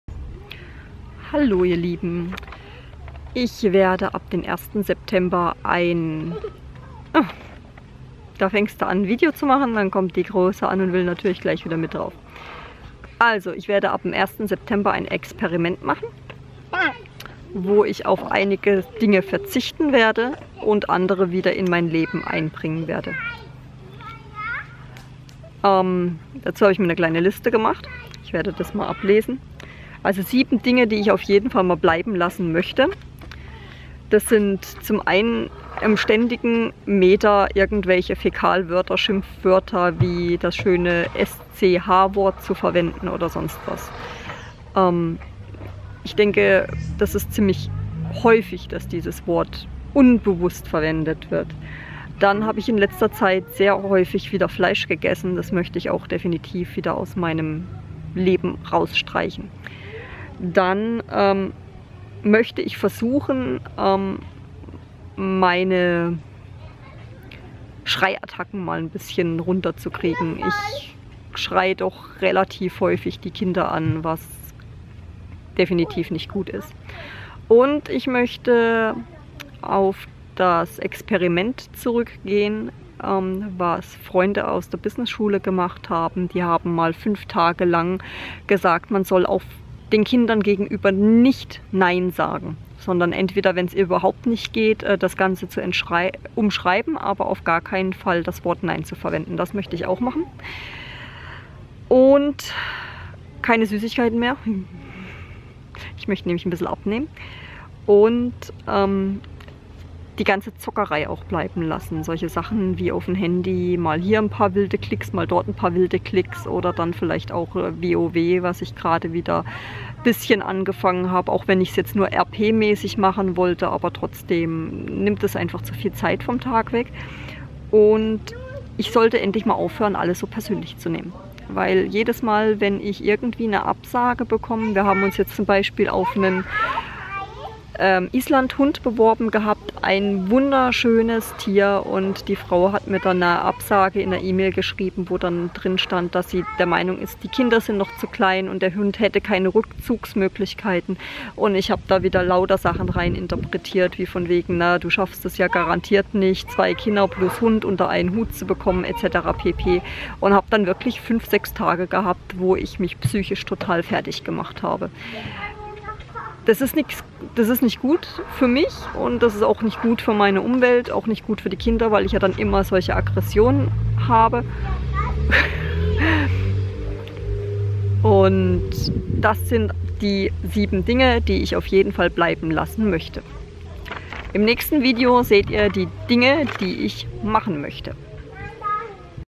Experiment! 7 negative Eigenschaften die ich ablegen möchte Teil I
Gedanken Hygiene
Das Experiment startet am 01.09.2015 und soll erst einmal einen Monat gehen.